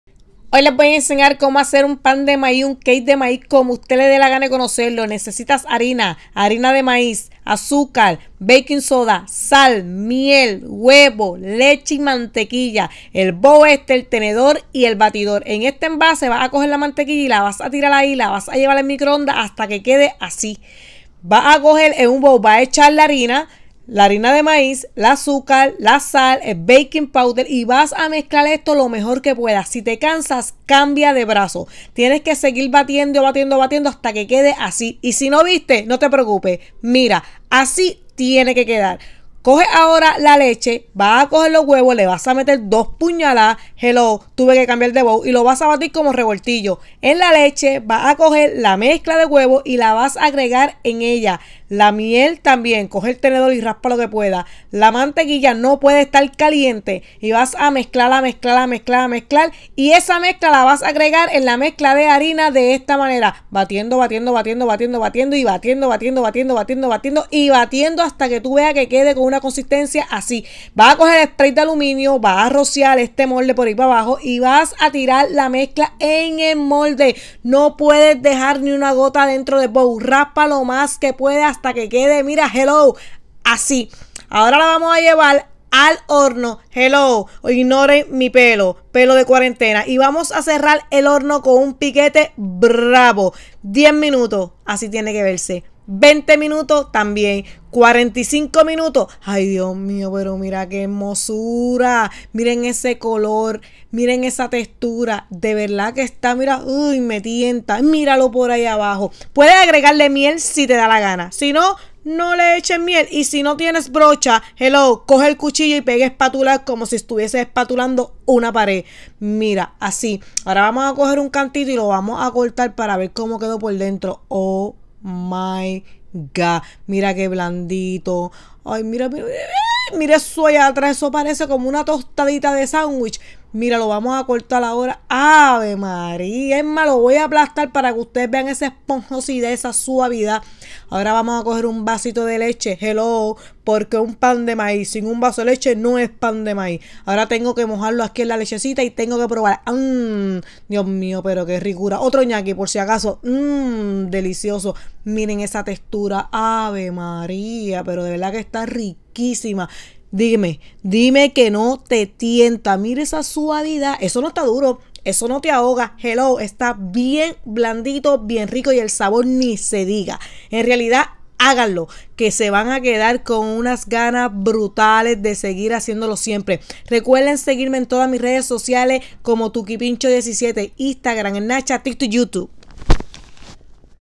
😱🚨🚨PAN D MAÍZ 🌽🚨vlog 7
UNNPAN SUPER FACIL
1 taza arina todo uso
1 taza arina d maíz fina
1/2 taza d azúcar
1 cucharada d BAKING POWDER ( en el video dije baking soda m equivoque)
1/2 cucharadita sal
1/4 taza miel
2 huevos
1 taza d leche
1/2 barra d mantequilla (ojo no puede estar caliente la mantequilla ok )